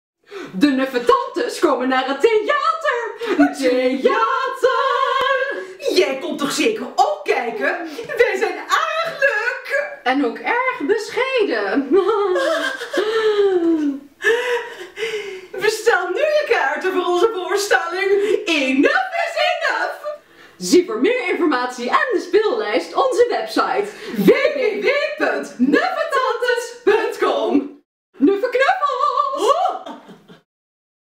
Muziektheater met Nuffe Tantes in Eenuf is eenuf in Myllesweerd
Zondag 7 januari 2018 staat er een muzikale en humoristische familievoorstelling op het programma van Theater Myllesweerd in Mill. Want dan spelen vanaf 14.00 uur de Nuffe Tantes de voorstelling Eenuf is eenuf.

Toegangstickets kosten 11,00 euro en dat is inclusief een consumptie. of haal ze op bij de balie van Myllesweerd.

Eenuf is eenuf

Wat betekent dat eigenlijk, Nuf? Kom er achter tijdens het verhaal van de ietwat ‘verknipte’ tweelingzussen Roosje en Greetje. Noodgedwongen hebben zij kapsalon den Groenerooij van hun vader overgenomen, met zijn vogeltje Jaune. Samen zitten de dames regelmatig met de handen in het haar en beleven ze een hoop rood-groene drama’s met de andere bewoners van Regenboogstad.
Zo heeft postbode Billie Blaauw niet veel zin om met één van de twee in het huwelijksbootje te stappen en hebben ze grote moeite huisbaas Naranja van Oranje de huur te betalen, omdat concurrente Pippa Paars alle klanten bij ze wegkaapt. Hopelijk zien ze door Rochee Rozé alles weer door een roze bril, want de dames hebben er genoeg van, “Eenuf is eenuf!”
Gespeeld en gecreëerd door de kleurrijke theatermaaksters Floor van der Poort en Irene Schouten. Met behulp van vernuftige kostuums, poppen, decors, liedjes en voice-overs van o.a. Karin Bloemen & Tony Neef, steken zij hun persoonlijke verhaal in een Nuf jasje: Durf te kiezen voor wat je écht wil! Daarnaast weten ze als geen ander op humoristische wijze in de huid te kruipen van de Nuffe Tantes.

Blijf op de hoogte van alle film-en theatershows in Myllesweerd. Like hiervoor de Facebookpagina, volg ze op Twitter en check regelmatig